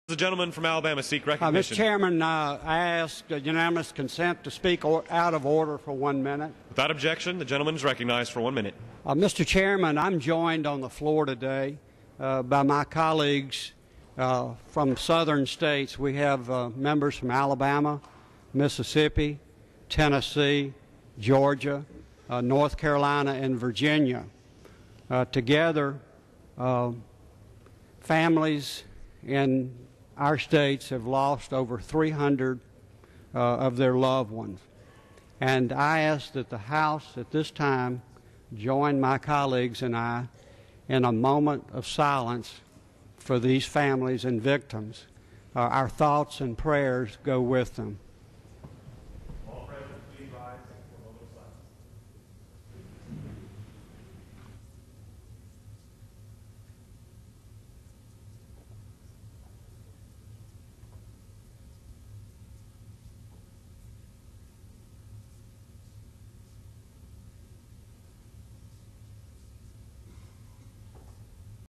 US House Holds Moment of Silence for Tornado Victims
On May 3, 2011, at 5:38pm (ET), the U.S. House of Representatives held a moment of silence for the victims of the violent storms and tornadoes of April 27 and 28.  Rep. Tom Graves (R-GA) stood in the well of the House chamber alongside other representatives of seriously affected areas.  Rep. Spencer Bachus (AL-06) spoke on behalf of everyone.